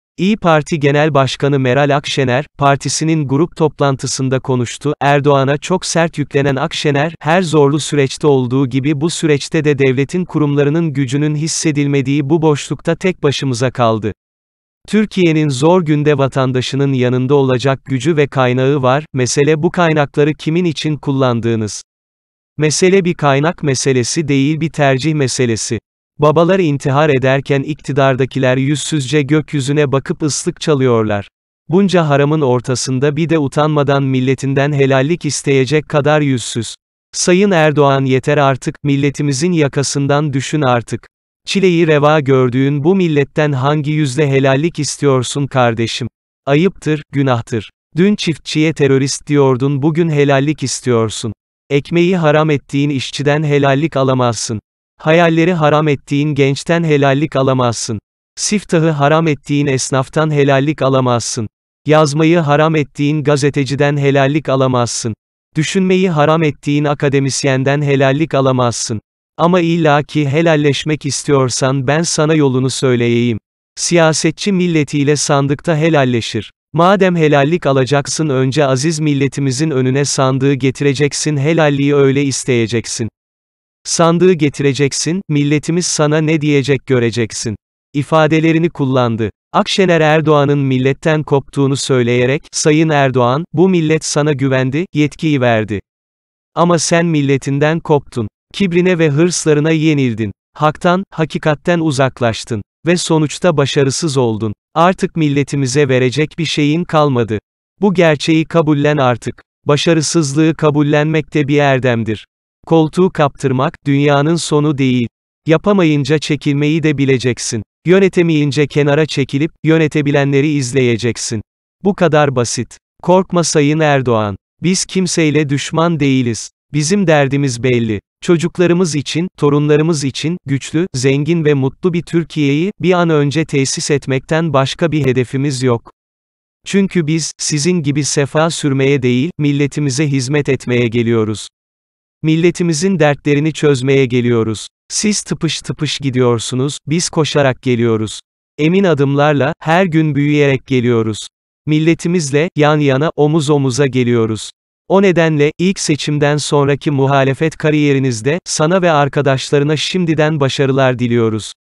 Akşener'den Erdoğan'a çok sert 'helallik' yanıtı
İYİ Parti Genel Başkanı Meral Akşener partisinin grup toplantısında konuştu. Cumhurbaşkanı Erdoğan'ı hedef alan Akşener "Hodri meydan sandığa gel" dedi.